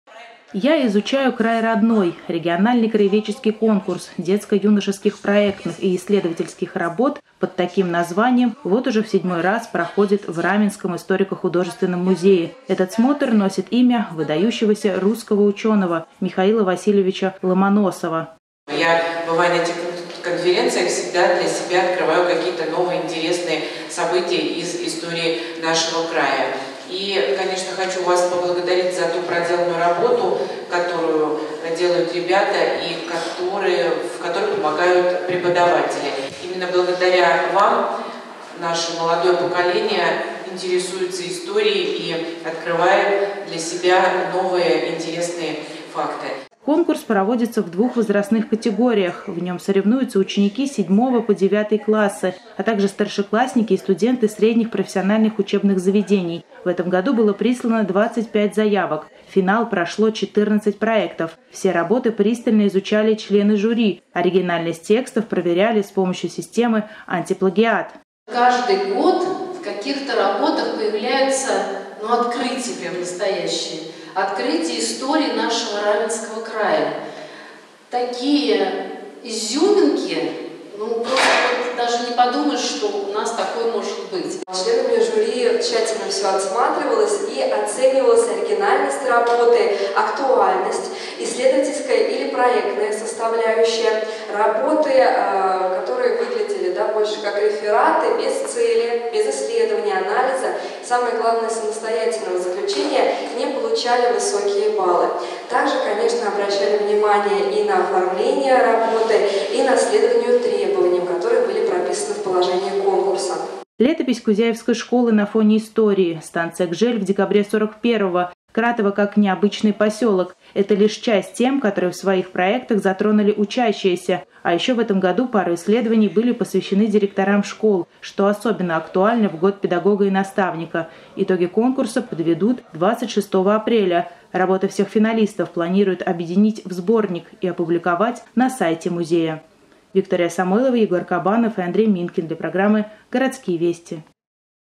«Я изучаю край родной» - региональный краеведческий конкурс детско-юношеских проектных.
«Я изучаю край родной» - региональный краеведческий конкурс детско-юношеских проектных и исследовательских работ под таким названием вот уже в 7-ой раз проходит в Раменском историко-художественном музее. Смотр проводится в двух возрастных категориях: в нём соревнуются ученики с 7-го по 9-ый классы, а также старшеклассники и студенты средних профессиональных учебных заведений. В этом году было прислано 25-ть заявок. В финал прошло 14-ть проектов. Все работы пристально  изучали члены жюри, оригинальность текстов проверяли с помощью системы «антиплагиат». Пару исследований были посвящены директорам школ, что особенно актуально в год педагога и наставника. Итоги конкурса подведут 26 апреля. Работы всех финалистов планируют объединить в сборник и опубликовать на сайте музея.